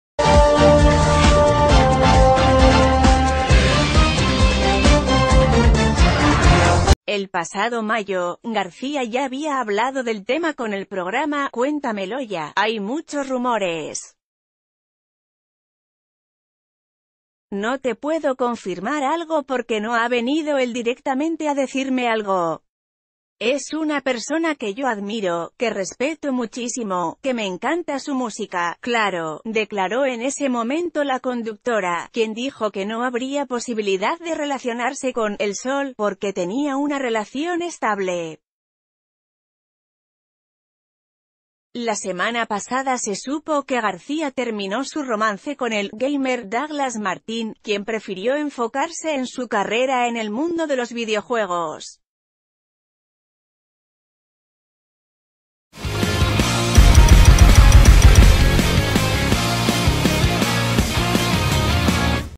Luis Miguel podría ser el nuevo novio de Yanet García "la chica del clima"
El pasado mayo, García ya había hablado del tema con el programa "Cuéntamelo ya"."Hay muchos rumores. No te puedo confirmar algo porque no ha venido él directamente a decirme algo. Es una persona que yo admiro, que respeto muchísimo, que me encanta su música, claro", declaró en ese momento la conduc...